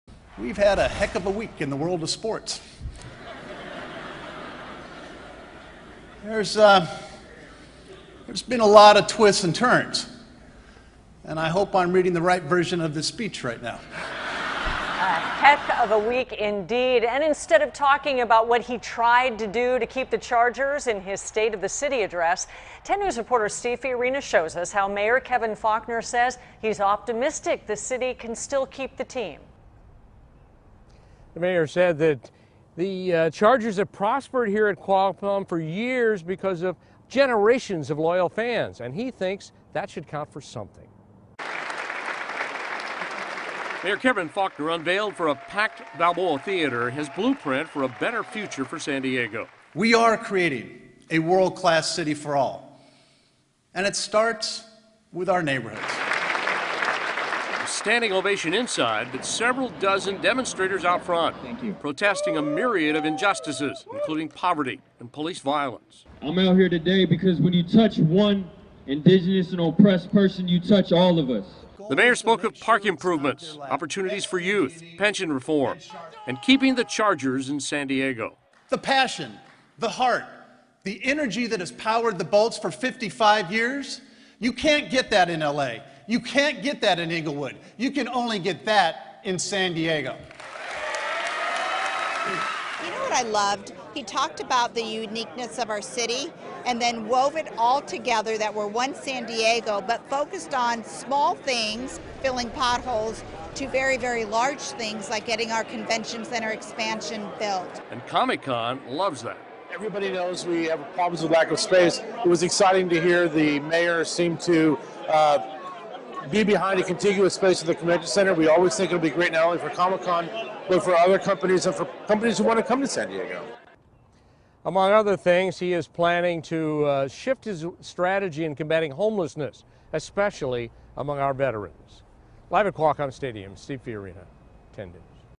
Faulconer in State of the City: Not too late to keep Chargers in San Diego
Mayor Kevin Faulconer said it's not too late to keep the Chargers in San Diego during his second "State of the City" address at the Balboa Theatre Thursday night.